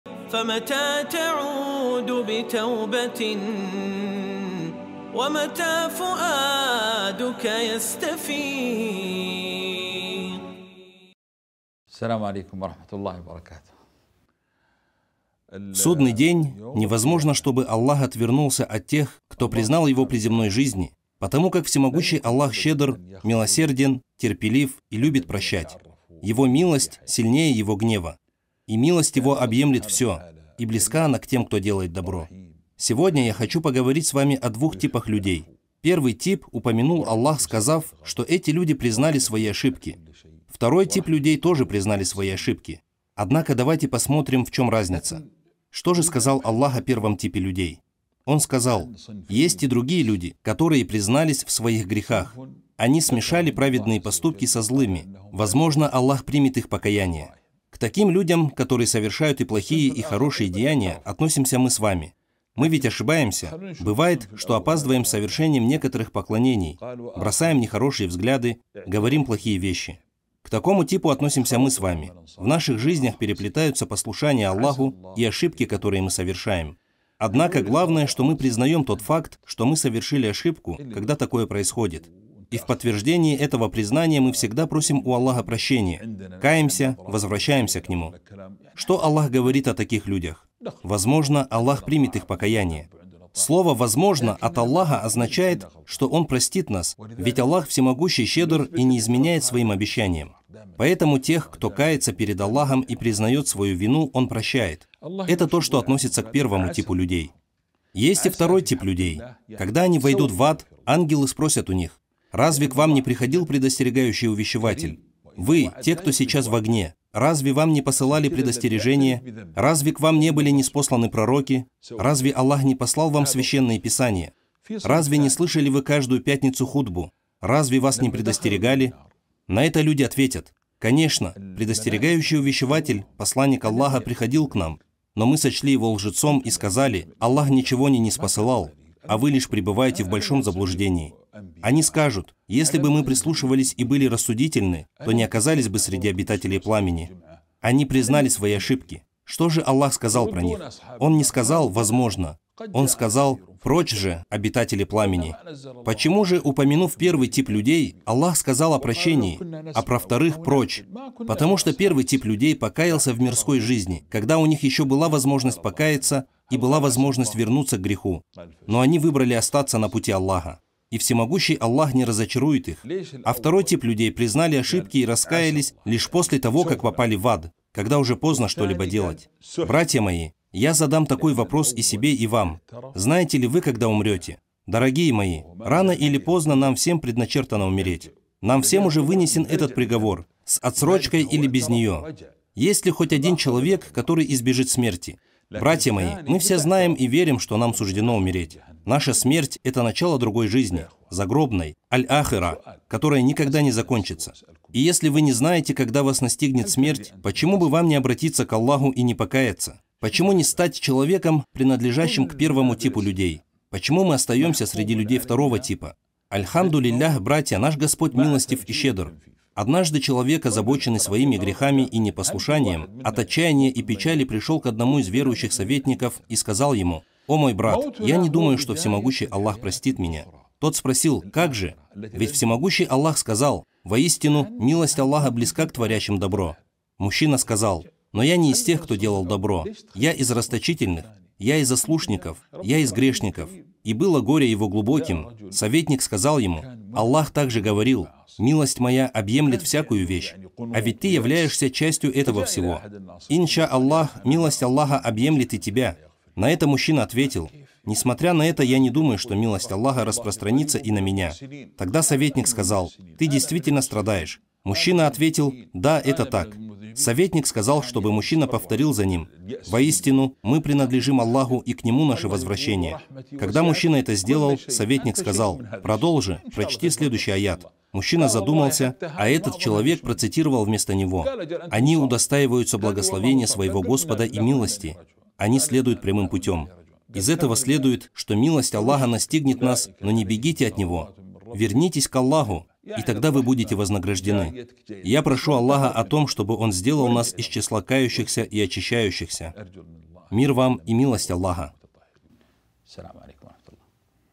Как Аллах встретит людей в День Суда? | АЛЛАХ НЕ ПРОСТИТ МЕНЯ... | Шейх АбдуРрахман аль-Бахили
ЮMoney:
Карта Мир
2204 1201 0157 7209

USDT: TCZoxfQzZYv9GLXkfAuFprQN59jgPbQNA6
Сеть TRC 20